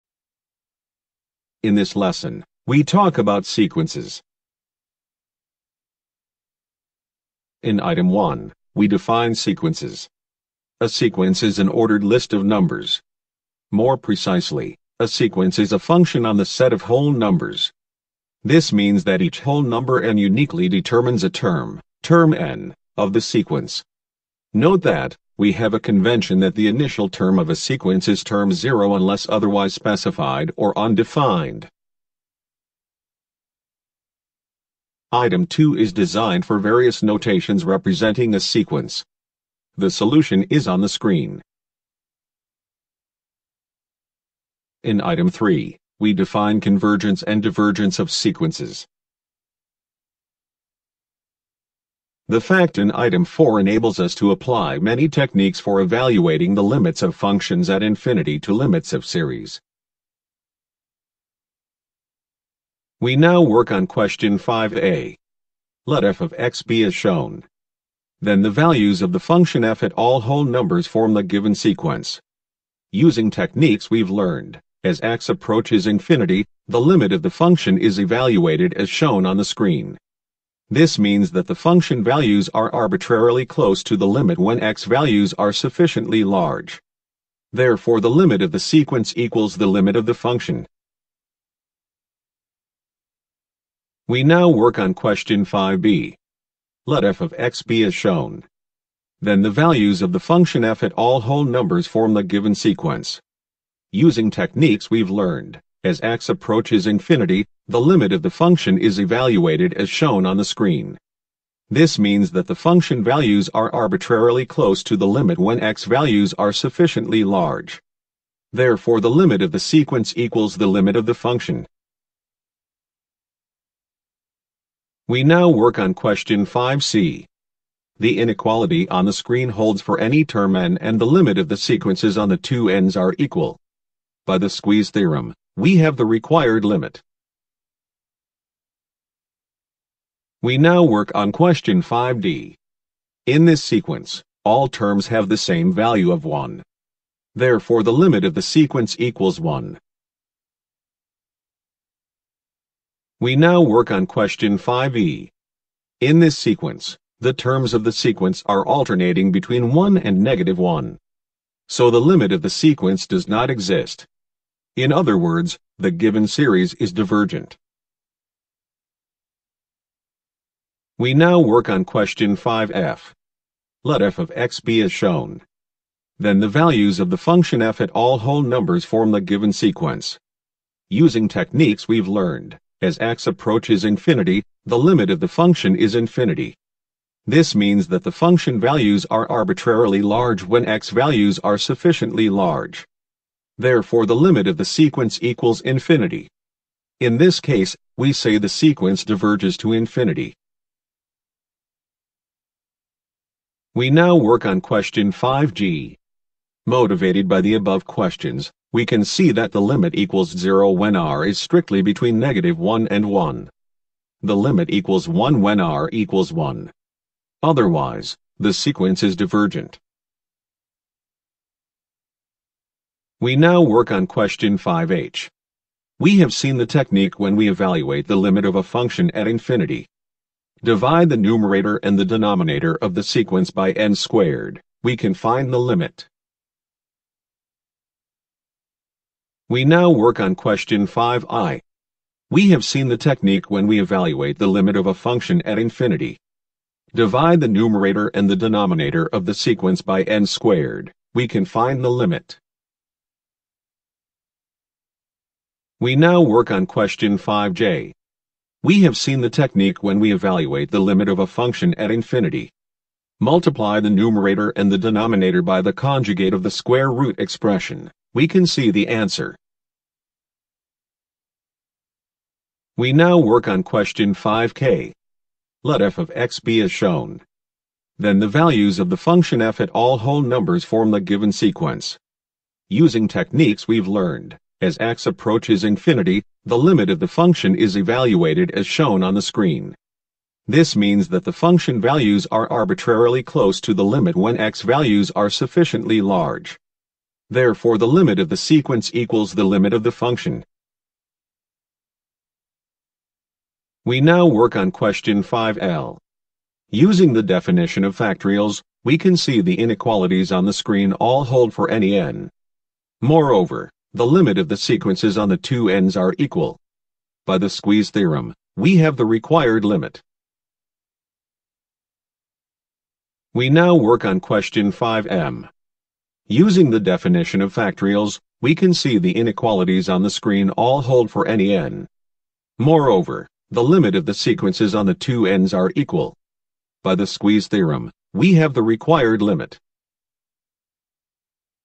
Sequences
This video covers the topic of sequences at a very fast pace. Please pause when needed.